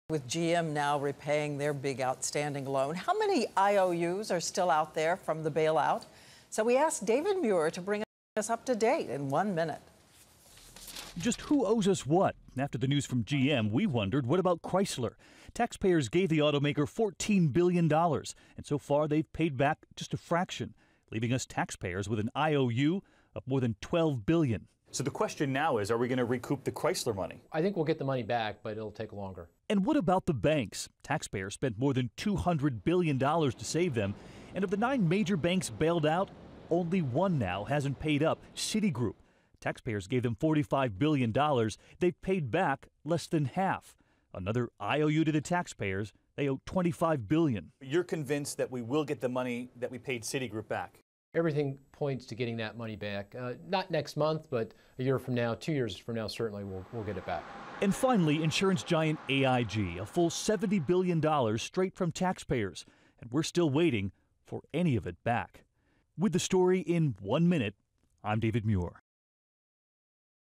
One-Minute Bailout Balances
David Muir checks up on how much bailed out companies still owe.